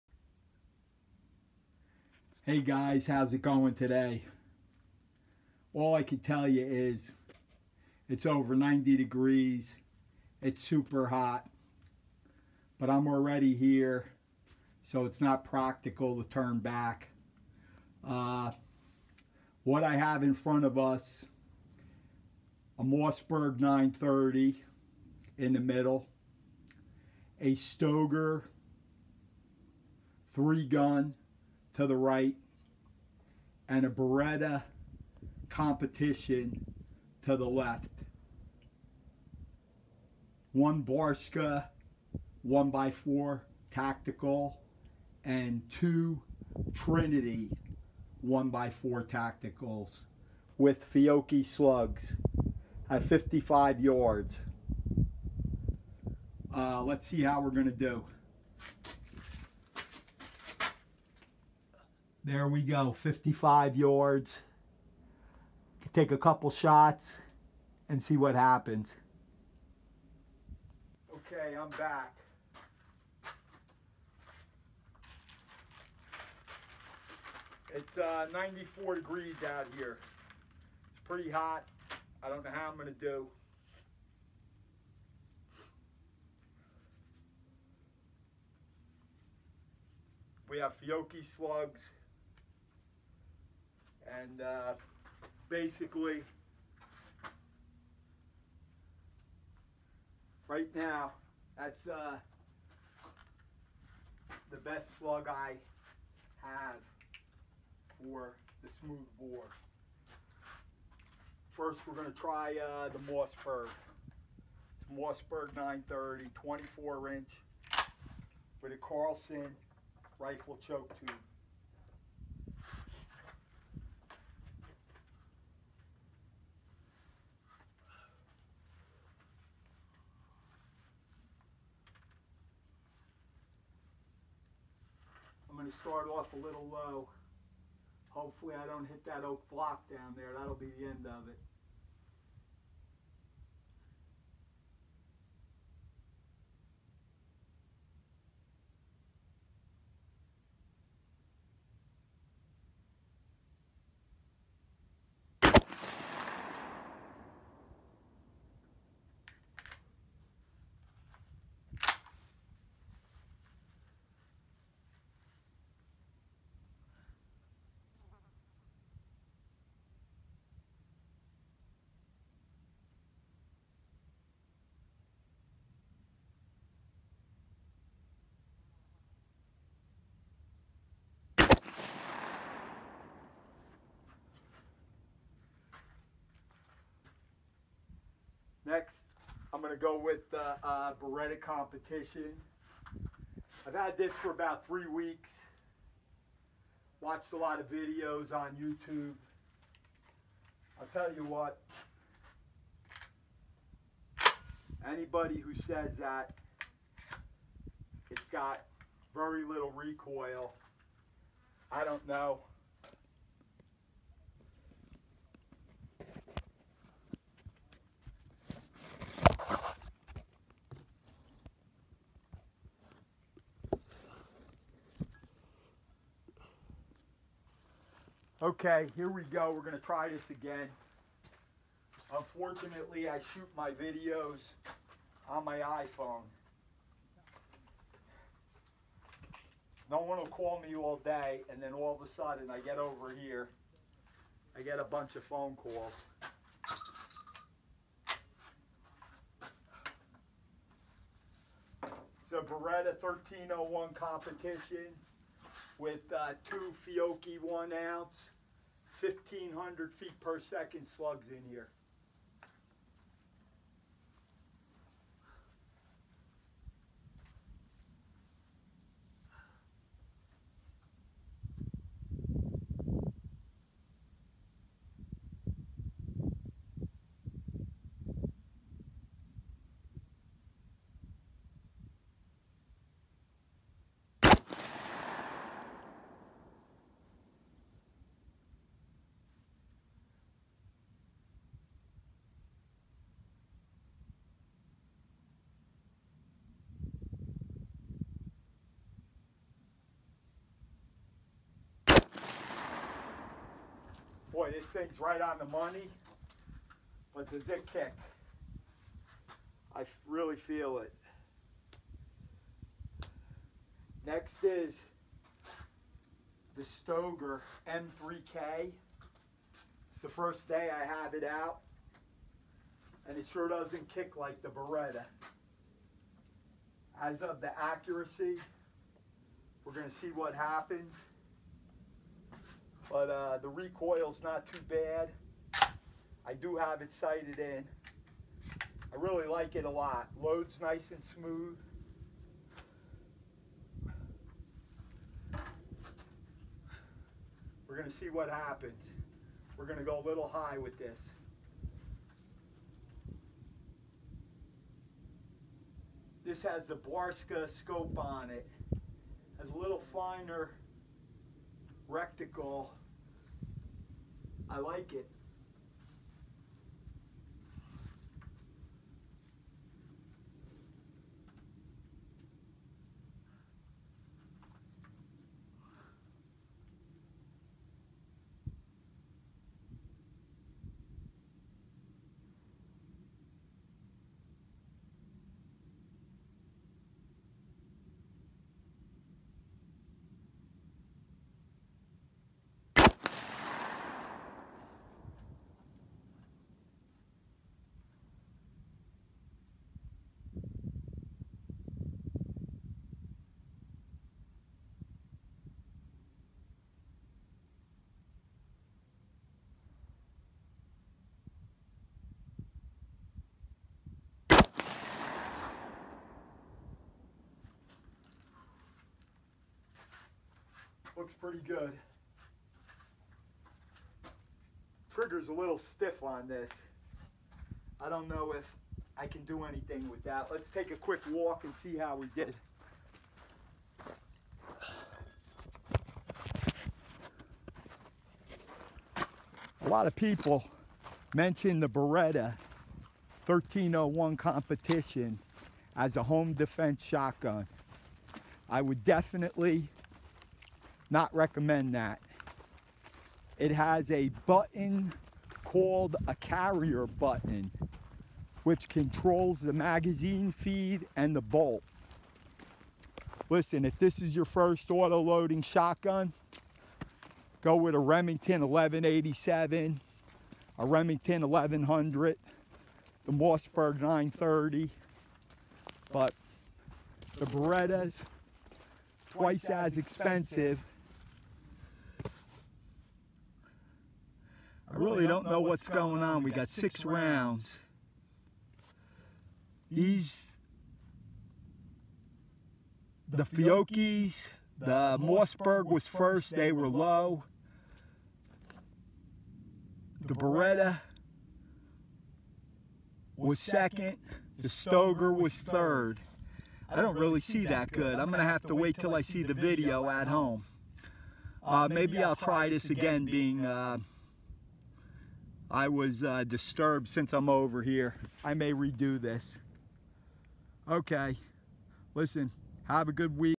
Shooting my Stoeger M3k, Beretta 1301 vs Mossberg 930 with Slugs
Shooting slugs @ 55 yards